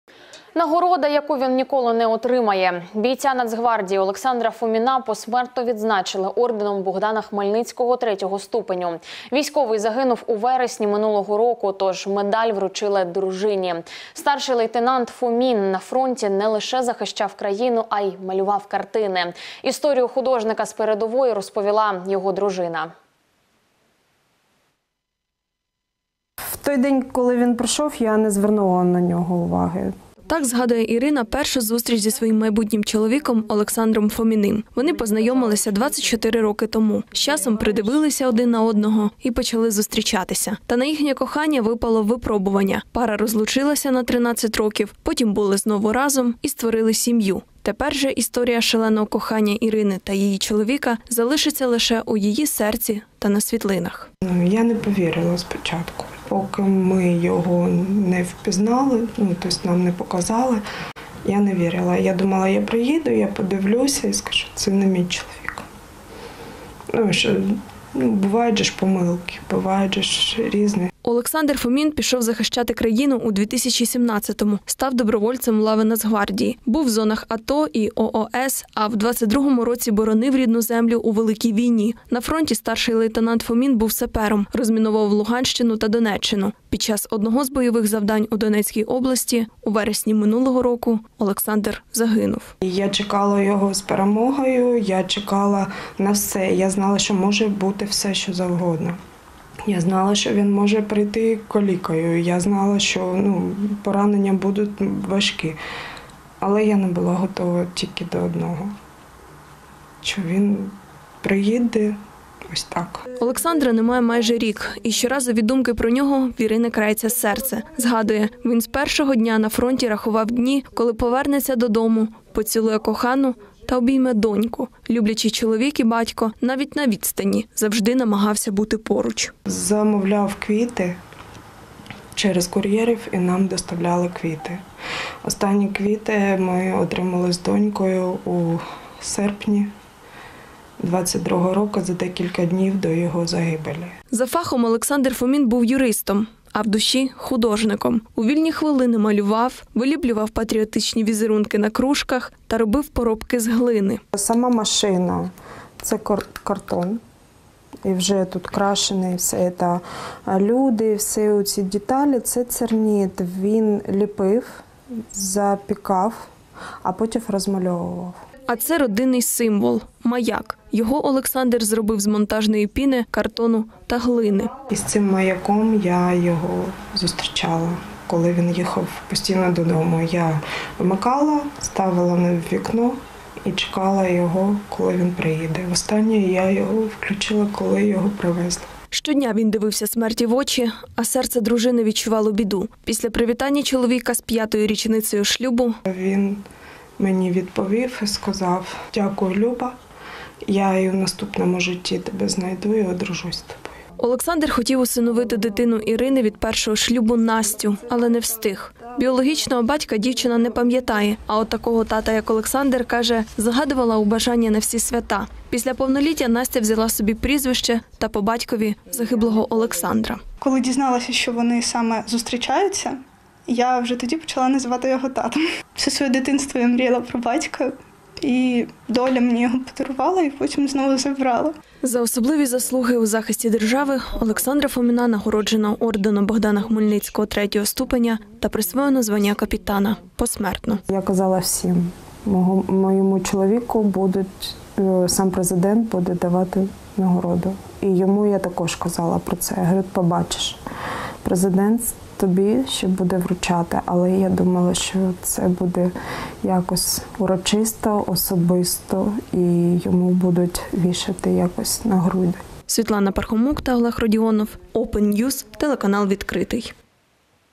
Військового з Дніпра Олександра Фоміна посмертно нагородили орденом Богдана Хмельницького
Художник з фронту. У Дніпрі посмертно відзначили бійця нацгвардії Олександра Фоміна